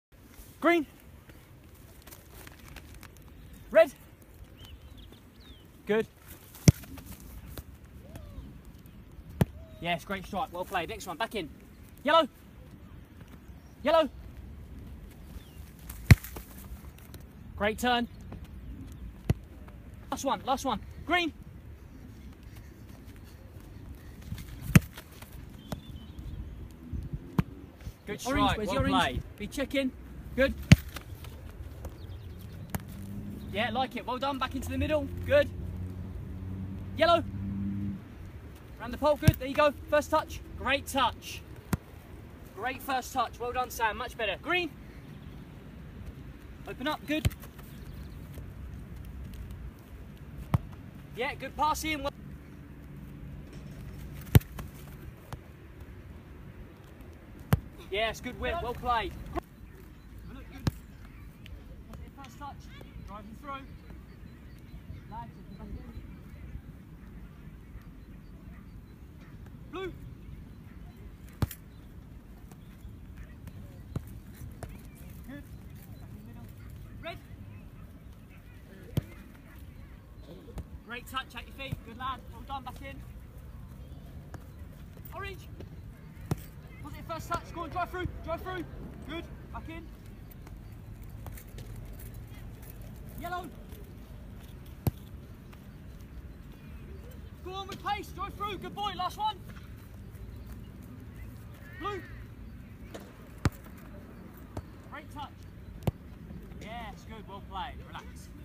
1-2-1 Football/Soccer Coaching
Footage from a 1-2-1 session with a client working on specific outcomes focusing on the following themes:

*Dynamic movement patterns and reactions
*Awareness and preparation prior to receiving
*Turning, adjusting and controlling skills